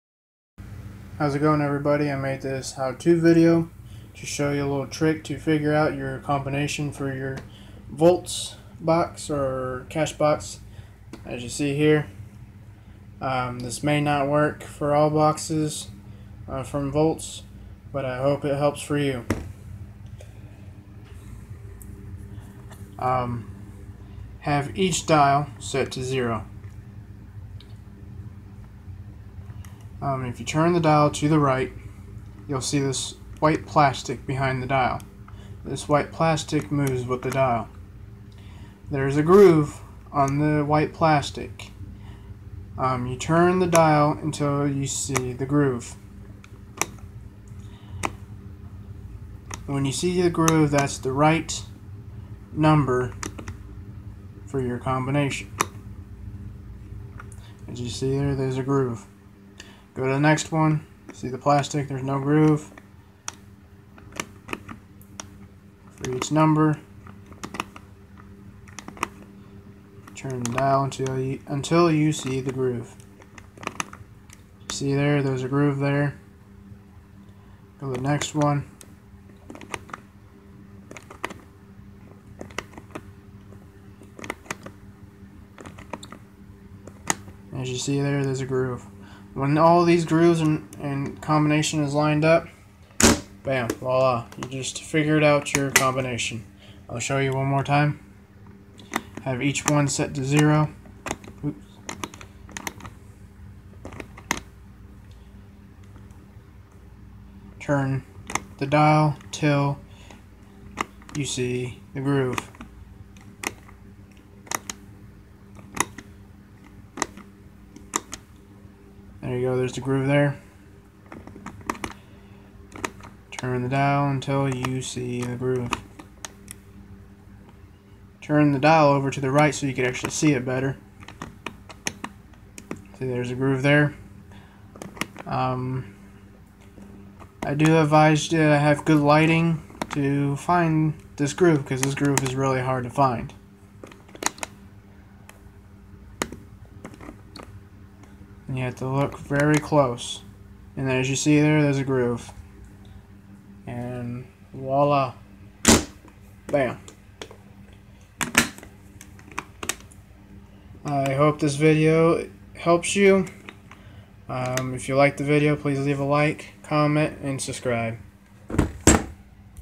How to unlock VAULTZ Cash/lock box! Older version/model.
Have you ever forgotten the combination to your lock box. Struggle no more here's how to unlock and find your combination to your lock box.